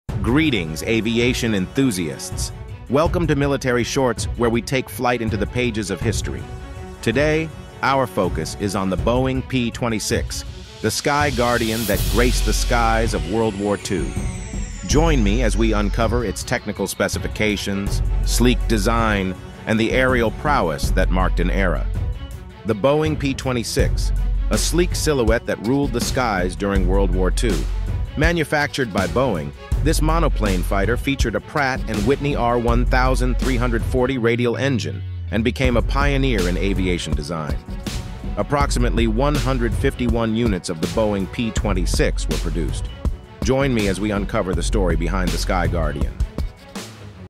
Part 1 - Sky Guardian: Boeing P-26 in WW2 | Military
Take to the skies with our latest Military Shorts episode! ✈️ In this brief exploration, we unveil the Boeing P-26, the Sky Guardian of World War II, and explore its technical specifications, sleek design, and the aerial prowess that marked an era. Ready for a flight into history? Join us as we soar with the Boeing P-26! 🌐